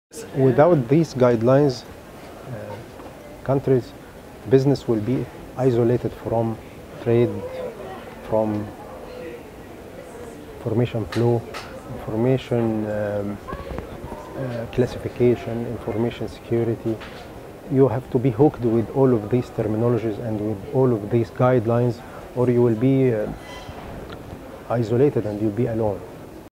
Global IGF 2017: Ziad Abdel Tawab on Standards for Security and Ethics
Ziad Abdel Tawab, director of Communications for Egyptian Government, shares their answer to the question, “In the next 10 years, will there be commonly accepted global guidelines for security and ethics for the Internet of Things, big data and artificial intelligence?” Recorded at the Global Internet Governance Forum 2017 in Geneva, Switzerland, by the Imagining the Internet Center, an initiative of Elon University, North Carolina, USA.